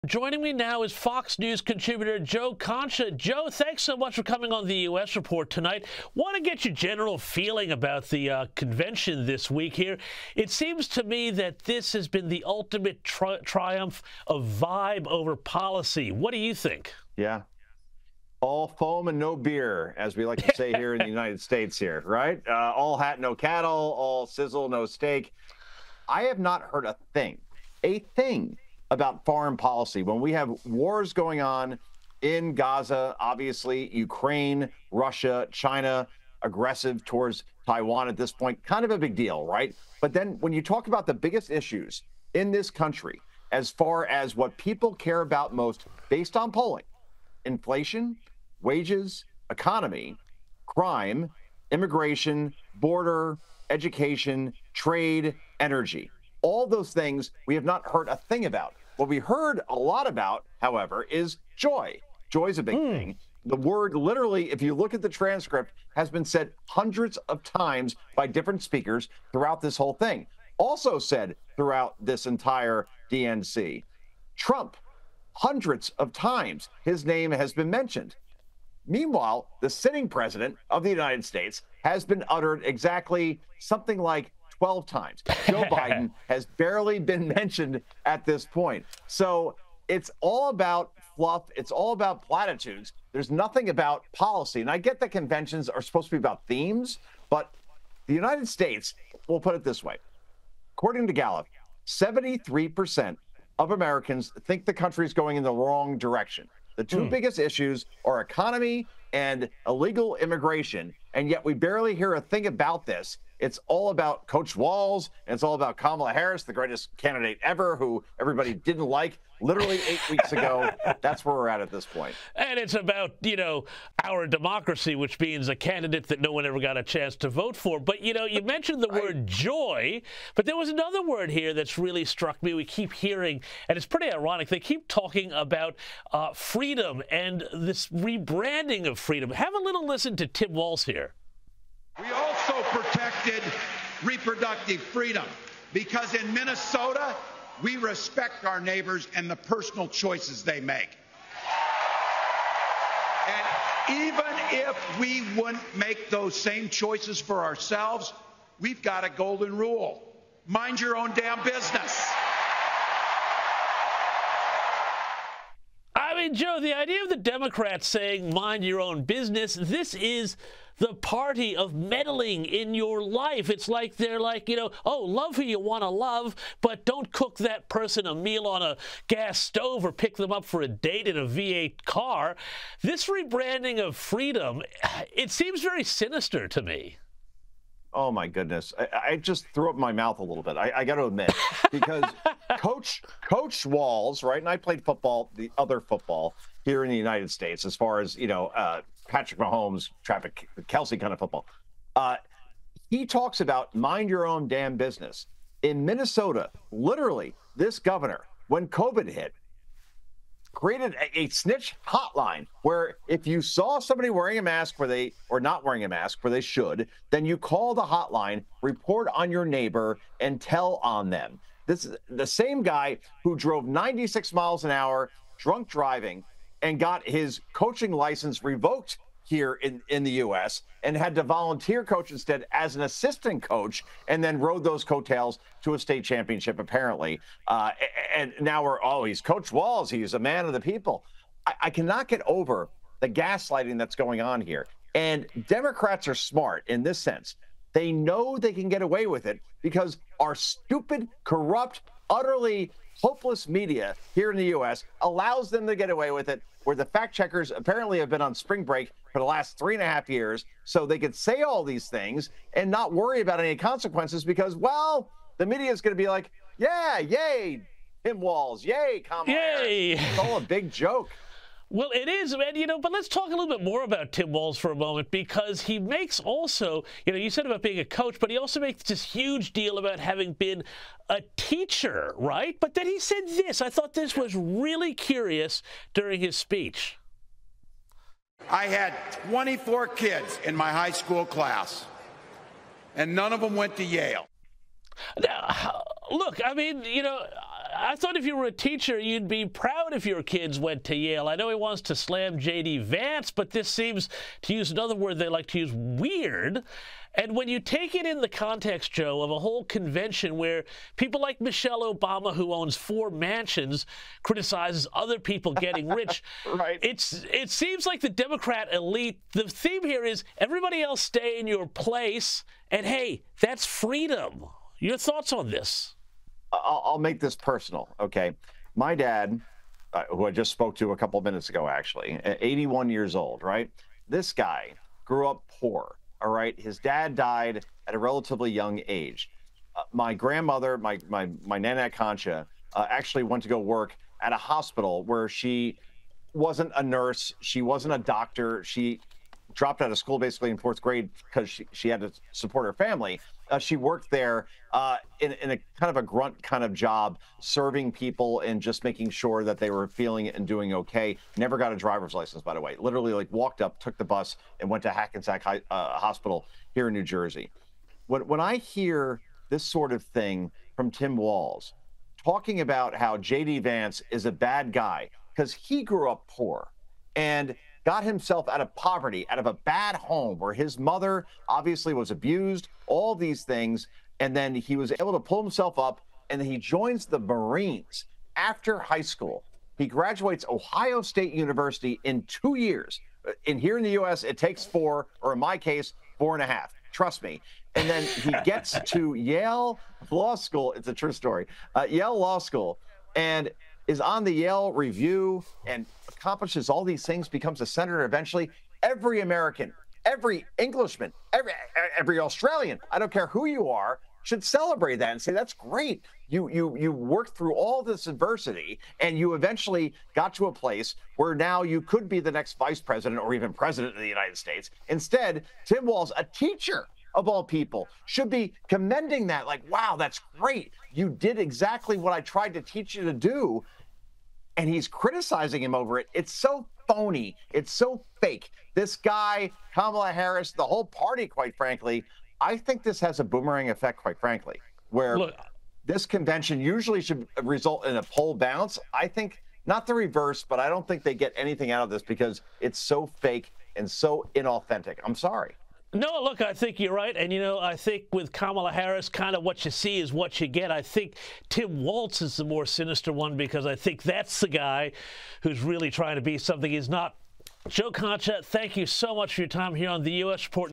‘All foam and no bear’: Joe Concha breaks down the DNC
Fox News contributor Joe Concha has branded the Democratic National Convention “all foam and no beer” with no major foreign or domestic policy announcements.

Mr Concha said the word “joy” has been uttered hundreds of times while President Joe Biden’s name has rarely been mentioned.

“It's all about fluff. It's all about platitudes. There's nothing about policy,” he told Sky News host James Morrow.